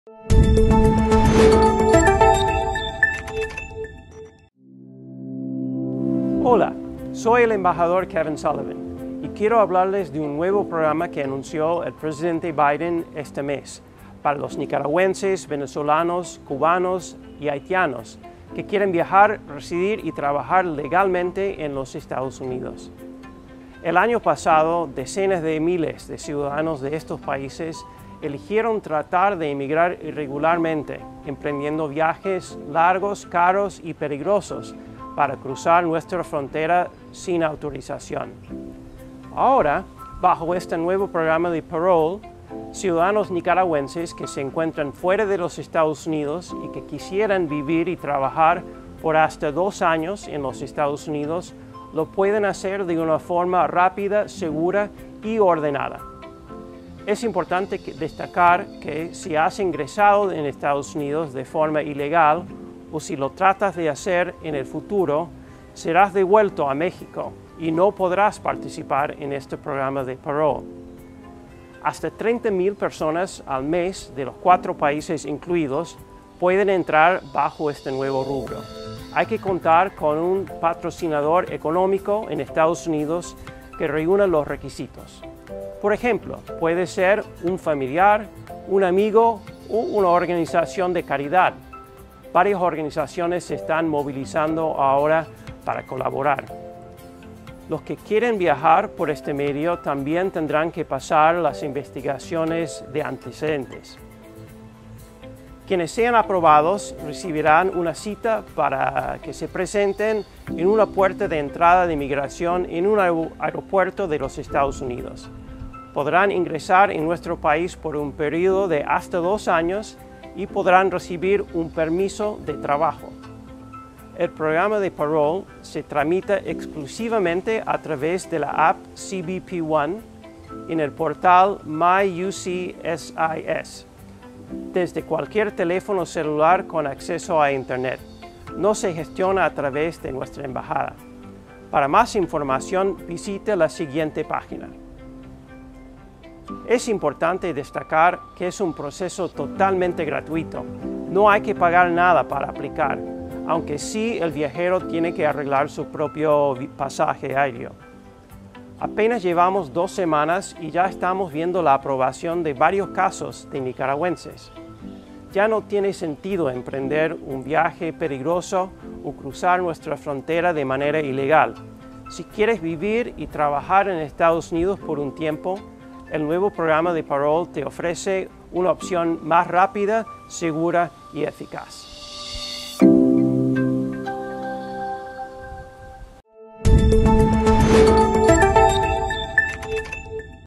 Embajador de EE.UU. en Managua explica que Parole Humanitario garantiza migración segura y ordenada
Los EEUU anunció un nuevo programa para que nicaragüenses puedan solicitar viajar, residir y trabajar en los EE. UU. de manera rápida, segura y ordenada.